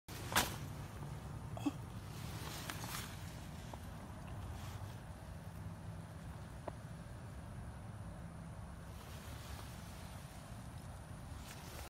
Collecting wild triops!
Thanks to the crazy weather we have another batch of wild triops in several locations on the property :)